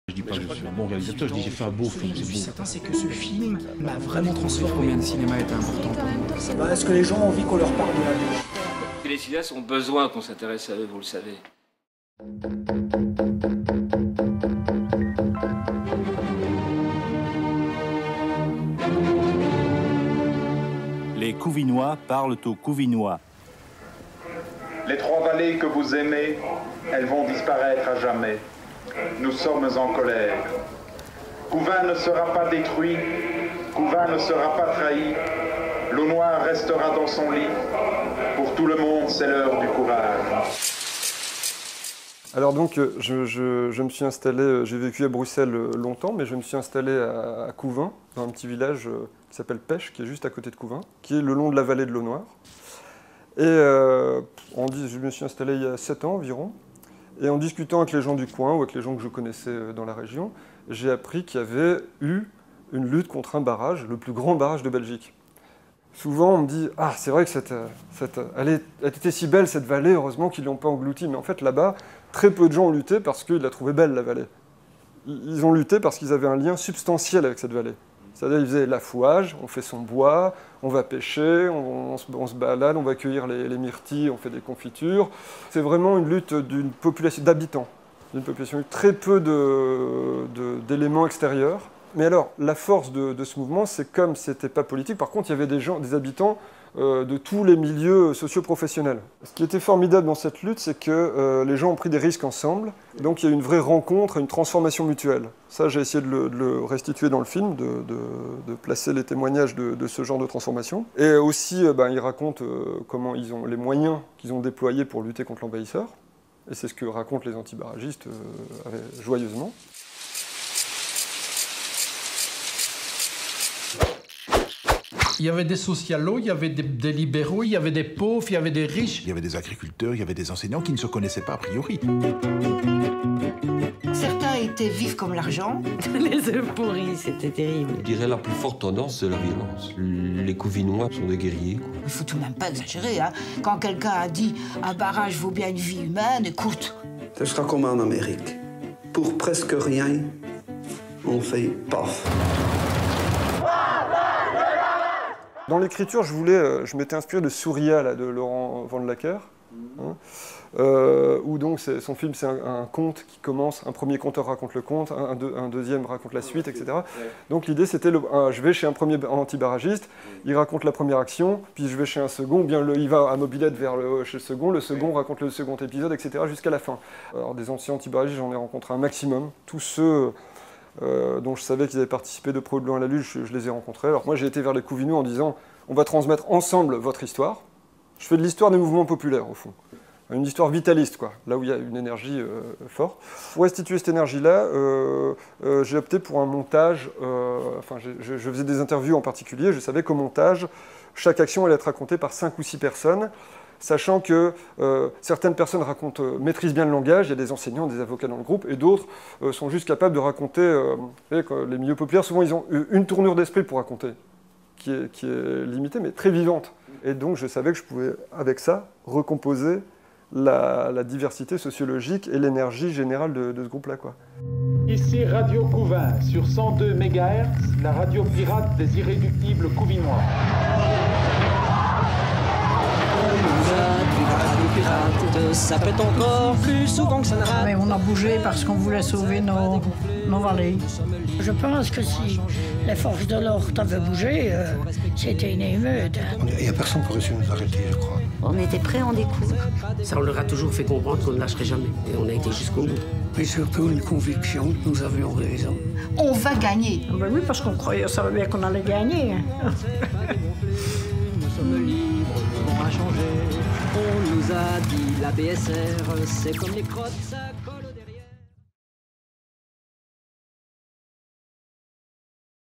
Rencontre avec Benjamin Hennot - "La bataille de l'eau noire"
Cinergie a rencontré Benjamin Hennot à l'occasion de la sortie toute proche de son nouveau documentaire "La bataille de l'eau noire".